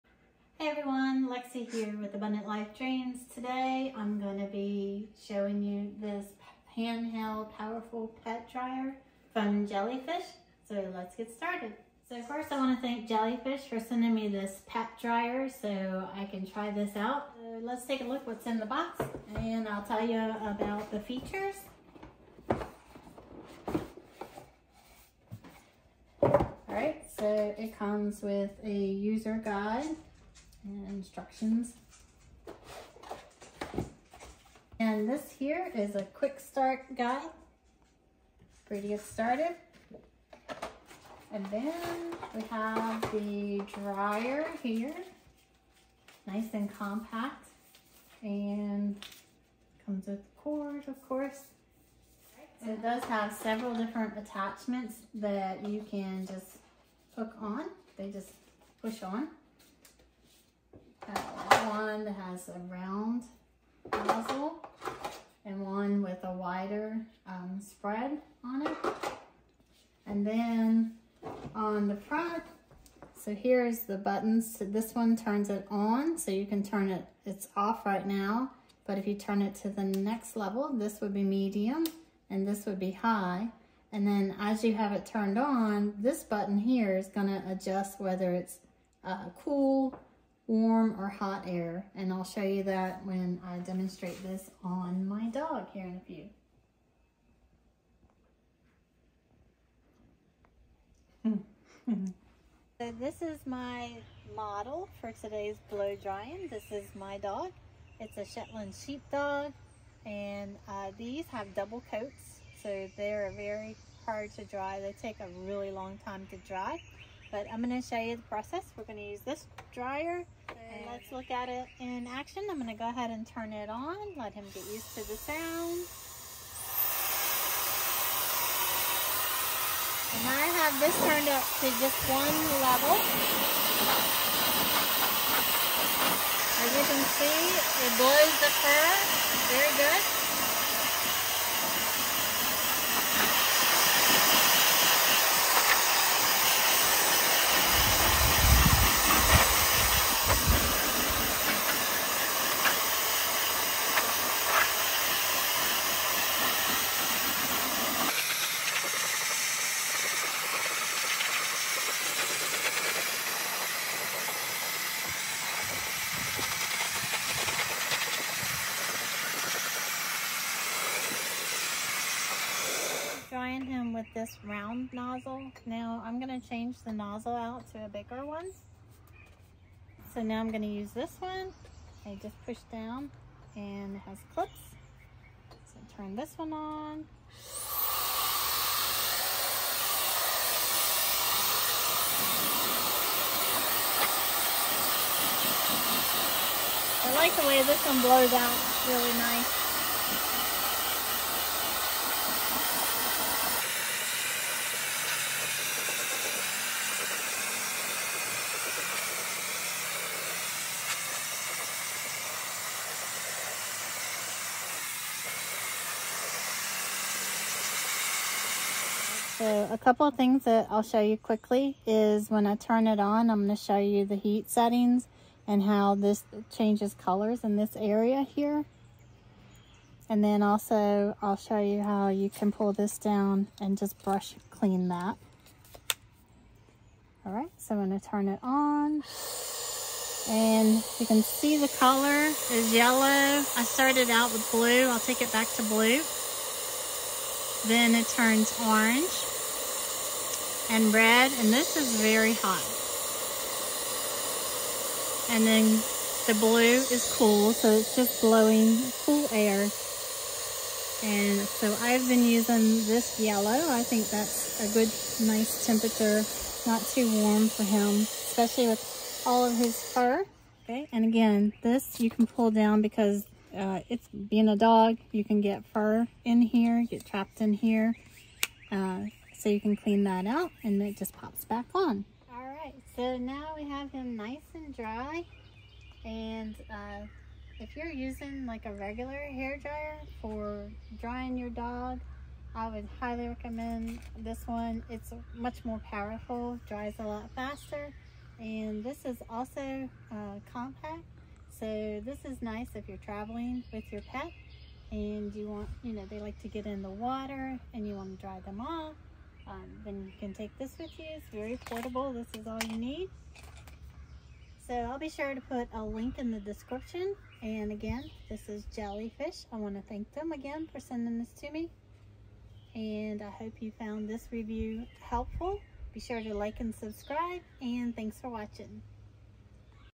JellyFish Dog Pet High Velocity Blow Dryer Portable Home and Travel Multiple Nozzles, Speeds, Temps
This Video is in collaboration with JellyFish

→LINK TO ALL MY S O C I A L  M E D IA AMAZON STORE AND FANS PAGES

 In this video, we introduce you to the game-changing High-Velocity Multi-Temp Dog Dryer. Say goodbye to wet and uncomfortable fur with this powerful and versatile tool that ensures a quick and efficient drying process for your furry friends.

Watch as we demonstrate the incredible features of the High-Velocity Multi-Temp Dog Dryer. With its high-powered airflow and multiple temperature settings, this dryer offers the perfect combination of speed and comfort. Whether your pup prefers a gentle breeze or a stronger gust, this dryer has got you covered!

The adjustable temperature settings allow you to cater to your dog's specific needs, ensuring their safety and comfort during the drying process. The high-velocity airflow helps remove excess moisture from the fur, preventing unpleasant odors, matting, and potential skin issues.

Join us as we showcase the ease of use and effectiveness of the High-Velocity Multi-Temp Dog Dryer. From small breeds to larger dogs, this versatile tool accommodates all sizes and coat types, making it a must-have for every pet owner and professional groomer.

Don't miss out on this informative video that highlights the convenience and benefits of the High-Velocity Multi-Temp Dog Dryer. Experience the joy of seeing your furry companion dry, happy, and ready for their next adventure!